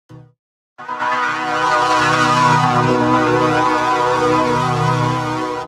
{2nd COLLAB ENTRY FOR AMLM859 |NE/P20} Windows Millennium Edition With Ruined Angelstone Chord
AMLM859, NE & P20 Stands For AzureMediaLogoMaker859, New Effects & Part 20

I will reveal the instructions once the "Windows Millenium Edition Effects" collab is released.

Please Contact & Join Me on MeWe!!!